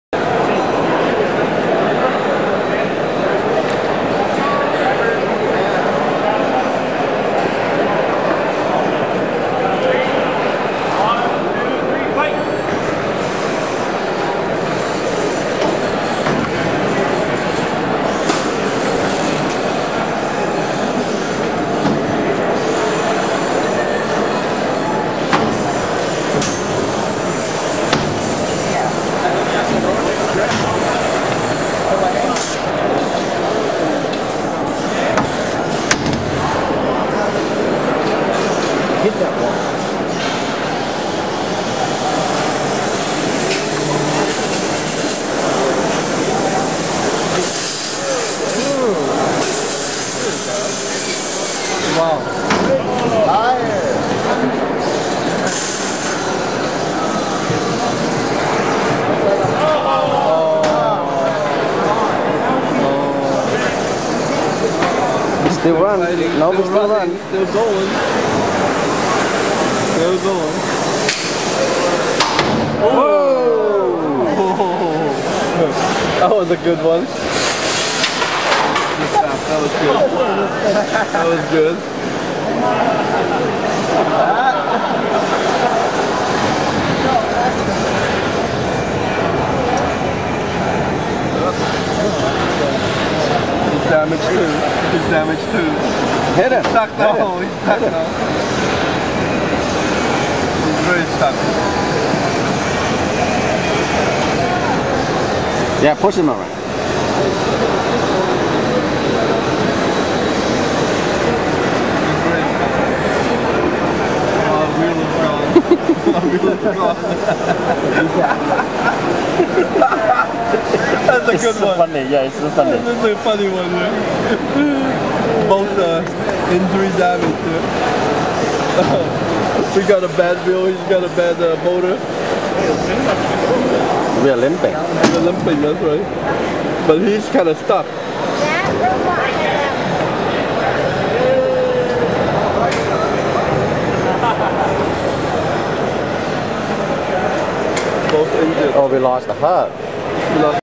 Poly-Stone vs Side Kick
Poly-Stone fights Side Kick at RoboGames 2007